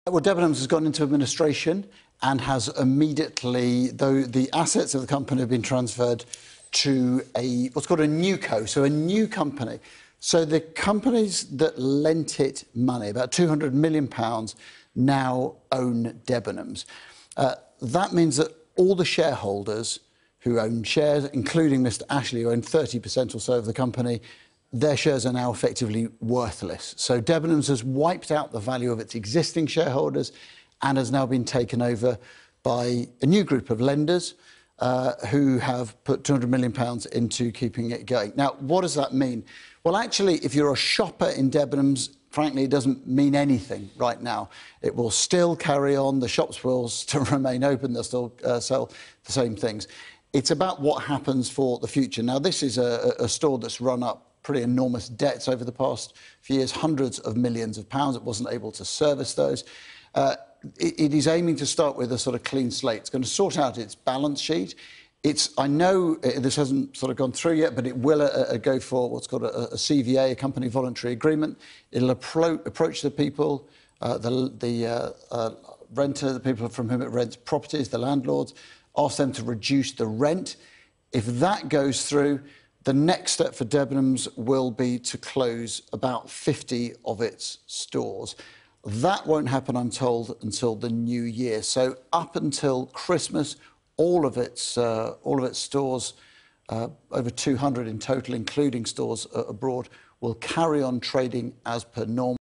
What does Debenhams' takeover actually mean?
Department store chain Debenhams has entered into a pre-pack administration and been taken over by its lenders.

Its chairman said in a statement: "Our customers, colleagues, pension holders, suppliers and landlords can be reassured that Debenhams will now be able to move forward on a stable footing."

Sky's Adam Parsons explains what this means.

and download our apps